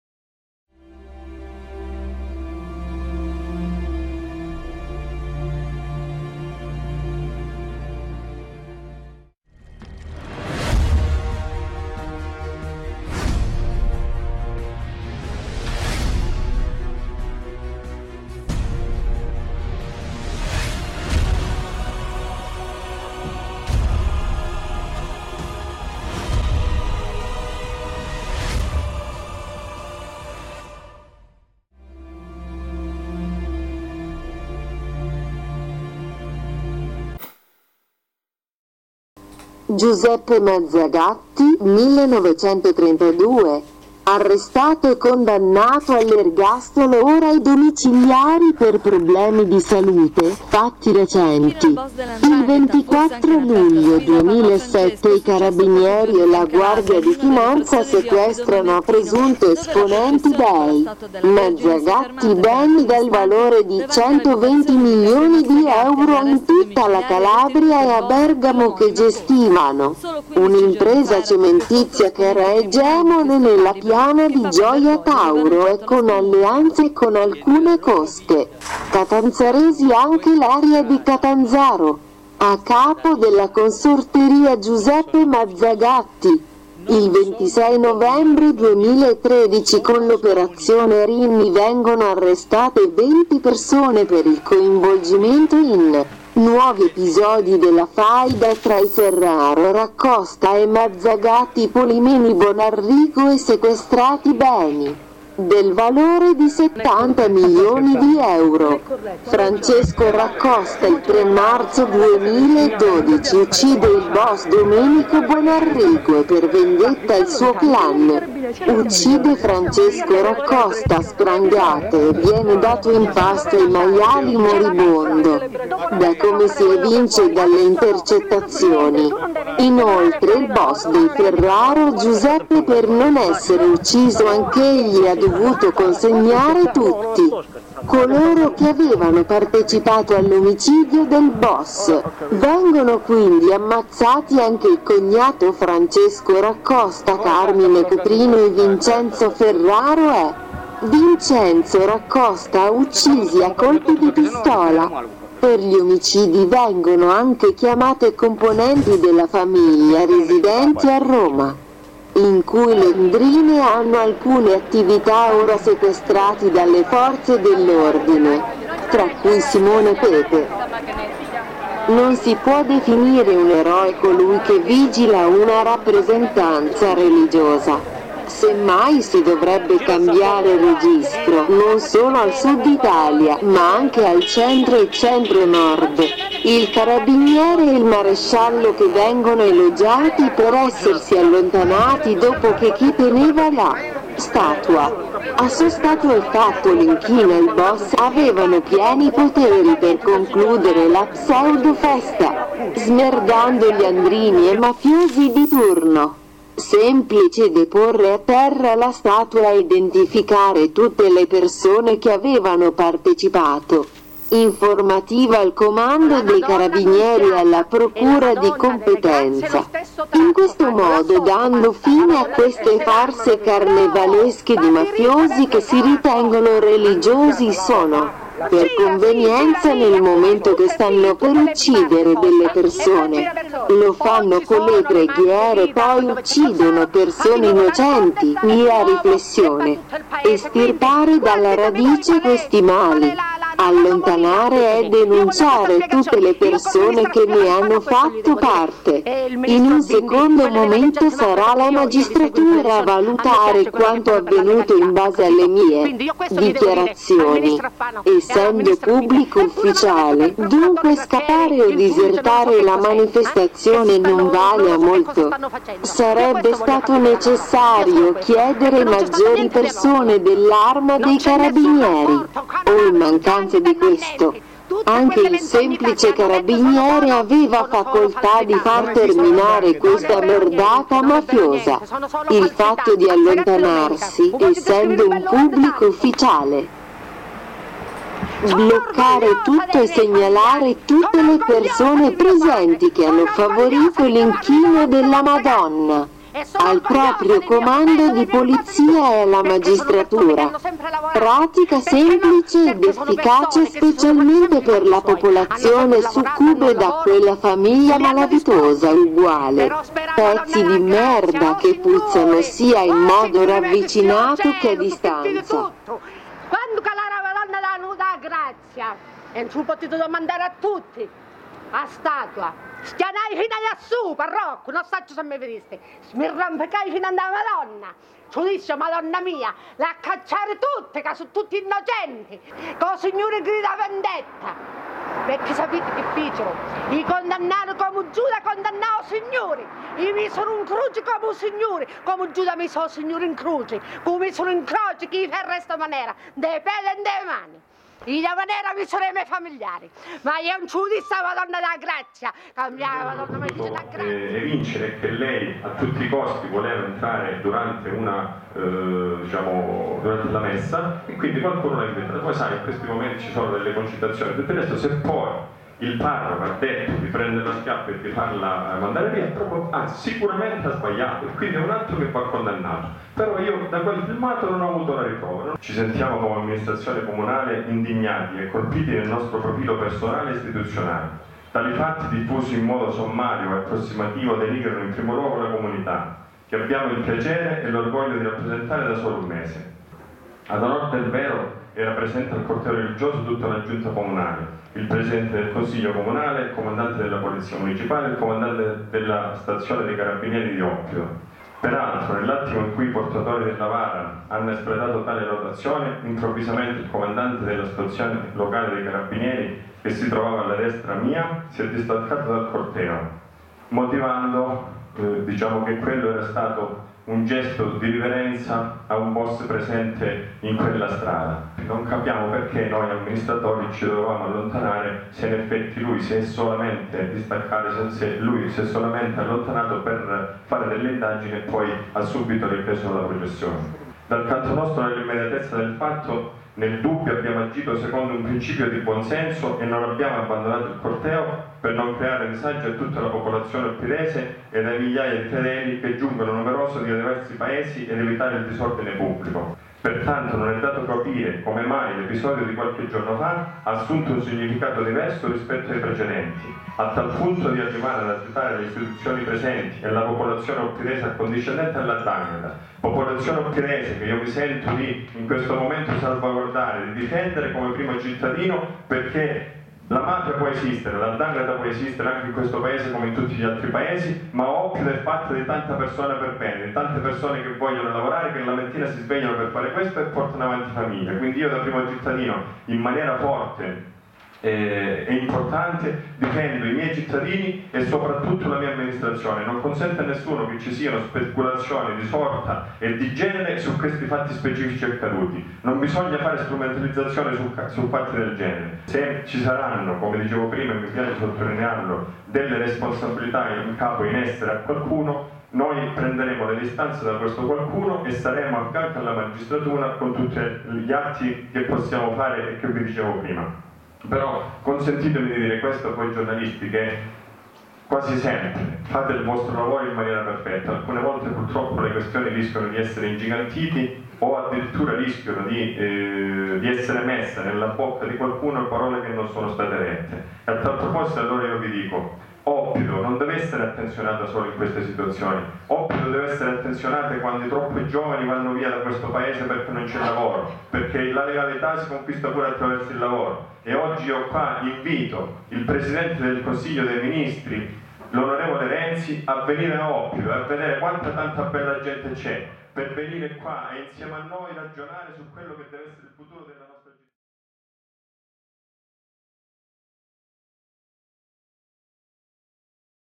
Oppido. Inchino della Madonna sotto casa del boss mafioso Giuseppe Mazzagatti
Non si può definire un eroe colui che vigila per lavoro in una manifestazione religiosa, semmai si dovrebbe cambiare registro (non solo al sud italia) ma anche al centro e centro nord. Il carabiniere e il maresciallo che vengono elogiati per essersi allontanati dopo che chi teneva la statua, ha sostato e fatto l'inchino al boss, avevano pieni poteri per concludere la pseudo festa, smerdando gli andrini e mafiosi di turno. Semplice deporre a terra la statua e identificare tutte le persone che avevano partecipato: informativa/denuncia nel comando dei carabinieri e alla procura di competenza, in questo modo dando fine a queste farse carnevalesche di mafiosi che si ritengono religiosi sono per convenienza nel momento che stanno per uccidere delle persone: lo fanno con le preghiere e poi uccidono persone innocenti. Mia riflessione: estirpare dalla radice questi mali. allontanare e denunciare tutte le persone che ne hanno fatto parte. In un secondo momento sarà la magistratura a valutare quanto avvenuto in base alle mie dichiarazioni, essendo Pubblico Ufficiale. Dunque scappare o disertare la manifestazione non vale a molto; sarebbe stato necessario chiedere maggiori persone dell'Arma dei Carabinieri, o in mancanza di questo, anche il semplice Carabiniere aveva facoltà di far terminare questa merdata mafiosa. Il fatto di allontanarsi, essendo un Pubblico Ufficiale, ha notevolmente sminuito le reazioni che il popolo del luogho si aspettava: bloccare tutto e segnalare le persone presenti che hanno favorito l'inchino della madonna, al proprio comando di polizia e alla magistratura. Pratica semplice ed efficace specialmente per la popolazione succube da quella famiglia malavitosa = pezzi di merda che puzzano sia in modo ravvicinato che a distanza.

* Realizzazione video di Lucio Musolino de IlFattoQuotidiano.